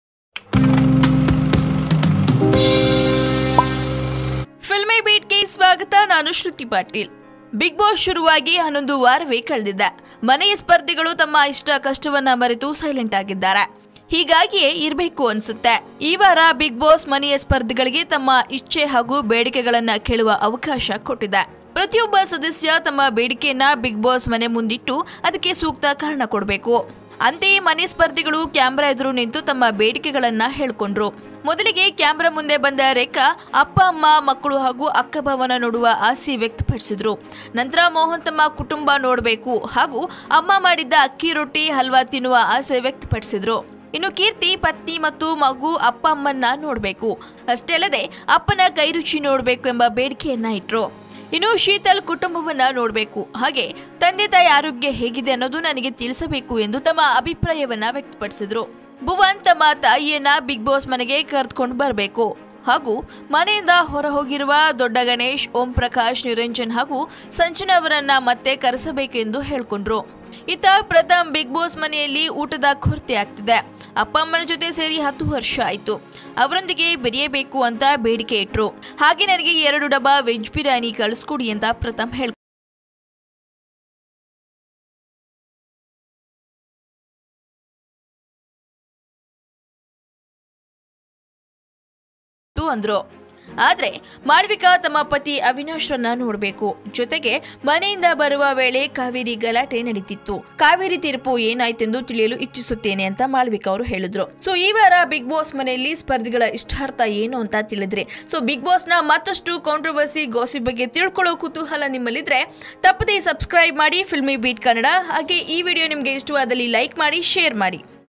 BiggBoss 4: Contestants And Their Demands | Filmibeat Kannada
Bigg Boss Kannada 4 Week 12 : Do you know why BiggBoss Contestants Are Really Happy yesterday .  For more information Watch this video